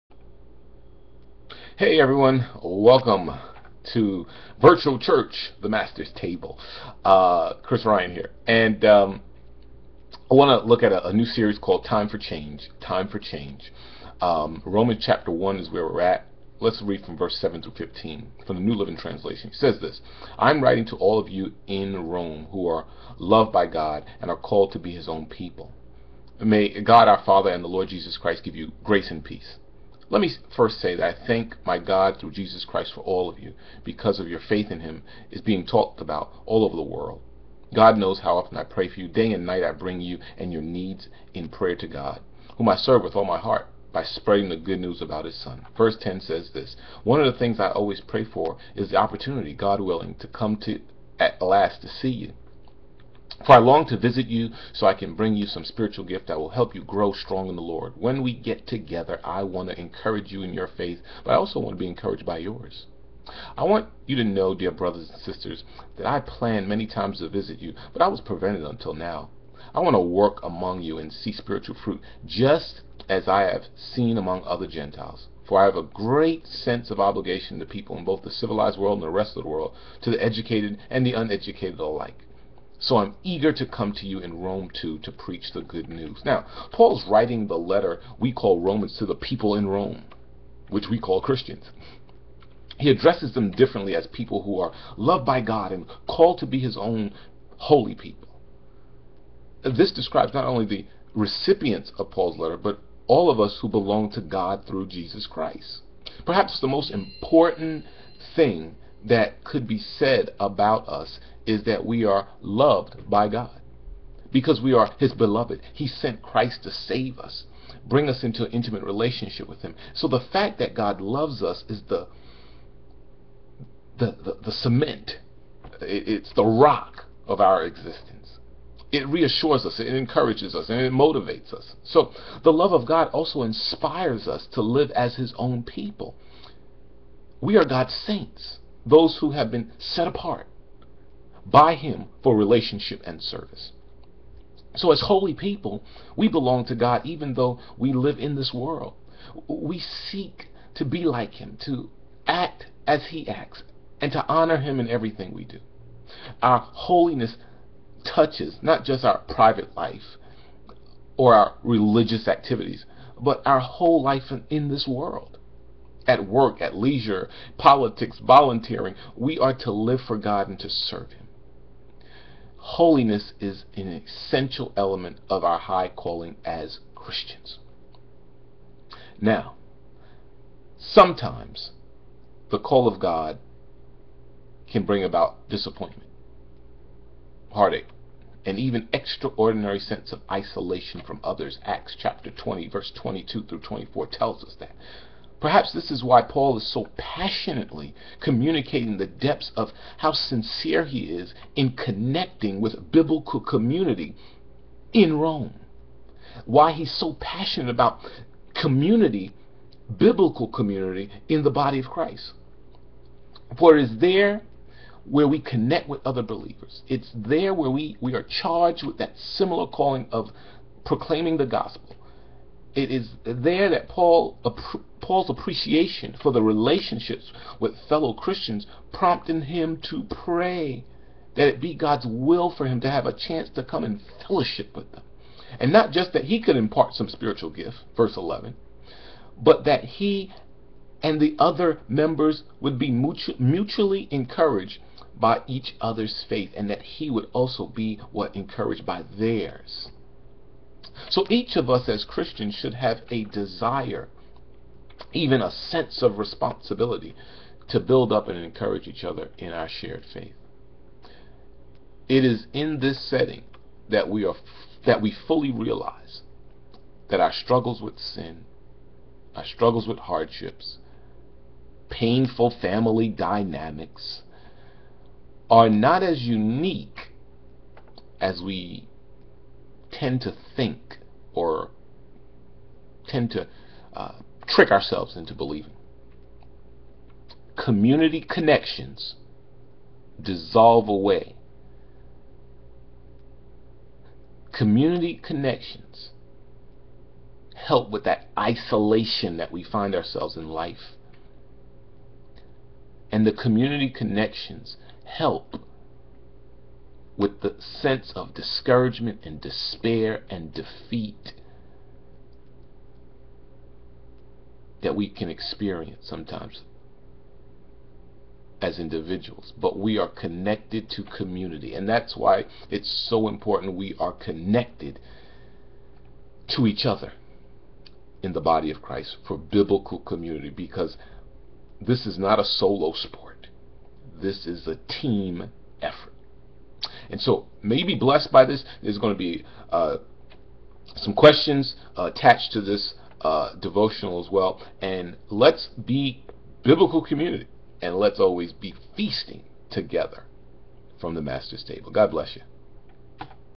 TIME FOR CHANGE- PART 1
Paul is writing the letter we call Romans to the people in Rome we'd call Christians. He addresses them differently, as people "who are loved by God and called to be his own holy people." This describes, not only the recipients of Paul's letter, but all of us who belong to God through Christ.